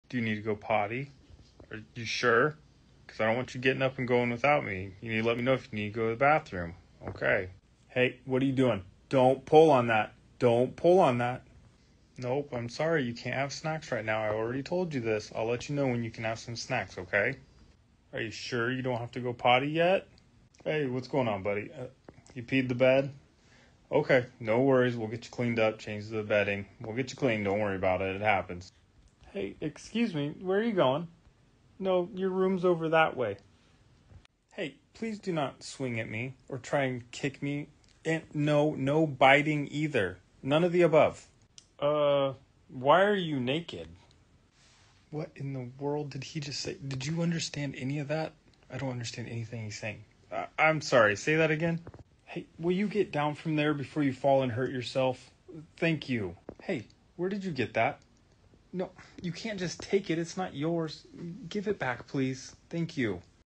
Talking to my toddler reminds me of talking to patients in nursing.
Talking to your toddler the same way you talk to patients in nursing. It’s amazing how the two overlap.